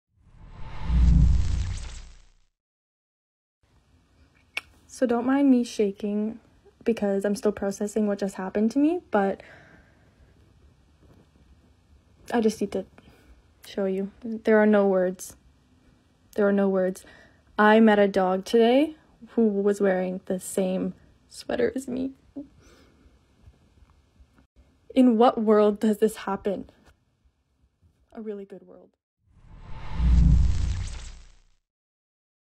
Woman Moved To Tears After Encountering Dog In Street Wearing Same Sweater As Her
This is the viral moment a TikToker tears up as she recounts meeting a dog in the street wearing exactly the same sweater as her.

Carmen MacLeod shared the video on her TikTok page 'Creatively Carmen Art', where she has 27,000 followers.

The clip, which is going viral, with 715,000 views at the time of writing, was accompanied with the message: "There are no words to describe what just happened, other than GOD IS GOOD."

Carmen, who is an artist and travelling muralist, told Newsflash that her sweater is by the brand Shien and that the dog she encountered was called Angel.

She said she was left "shaken" after coming across the pooch with the same sweater design as she had on.

Carmen says in the video: "So don't mind me shaking because I'm still processing what just happened to me, but I just need to show you, there are no words.

"I met a dog today who was wearing the same sweater as me. In what world does this happen?"

She tearfully adds: "A really good world."

The image shows Carmen and Angel wearing matching blue jumpers with fluffy white clouds on them.

Netizens flooded Carmen's comments section with remarks about the unlikely encounter, with TikTok followers telling her "that's your soulmate" and that she "officially lives in a Disney Pixar movie".

Another TikTok user remarked: "My dream encounter!"

Carmen said in a comment: "The owner and I are even getting coffee together."

Market research consultants Million Insights said that the dog clothing industry is expected to reach USD 16.6 billion by 2028.